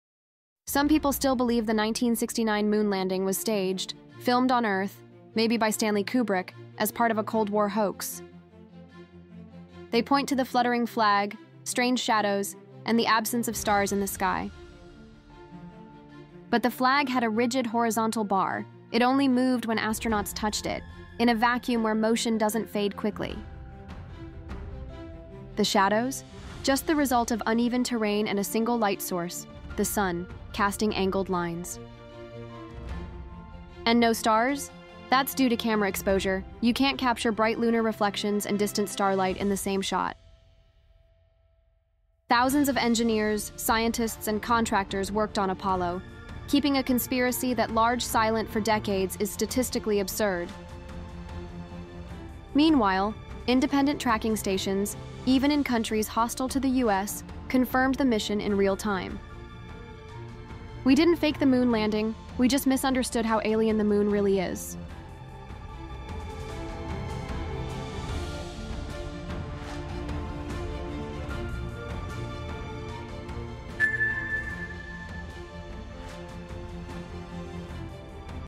Did We Fake the Moon Landing? Debunking the Myth with Logic | ESL Explainer
Was the Moon landing a hoax? Some say the flag moved, the shadows are off, and there are no stars. In this quick explainer, we debunk the conspiracy theory using real science, camera physics,  logic, and accessible English.

🌍 Explore more Shorts on science, conspiracies, and critical thinking.
📚 Improve your English with real-world topics — perfect for ESL learners.